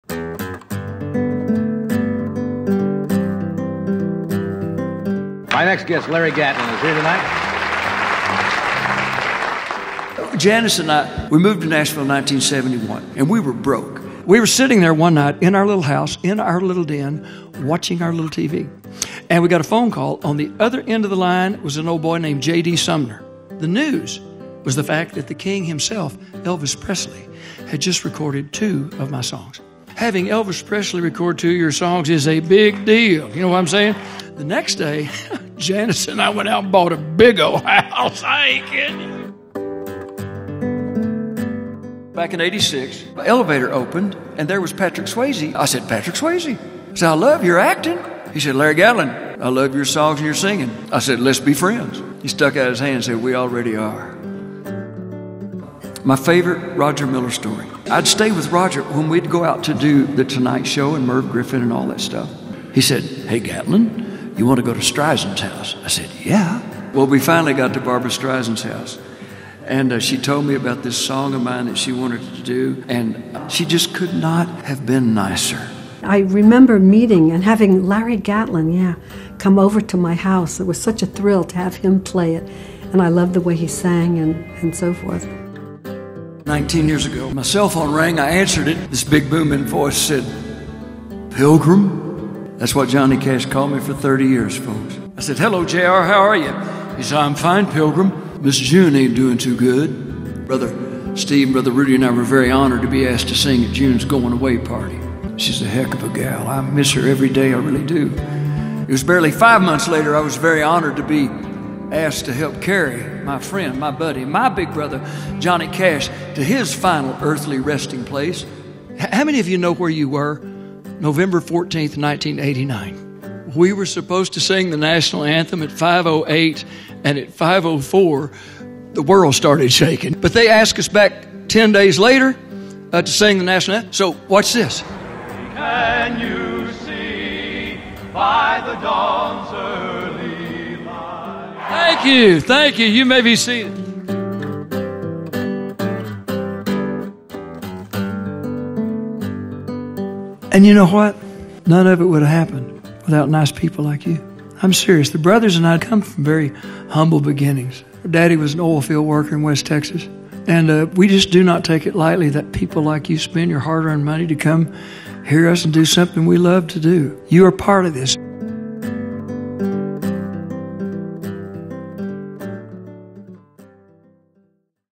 Larry Gatlin | Sizzle Reel
Grammy-winning country singer Larry Gatlin was born in Seminole, Texas, the son of a driller in the oil field. The oldest of the three Gatlin brothers, by age six, he was already accompanying younger brothers Steve and Rudy in singing at family and church events.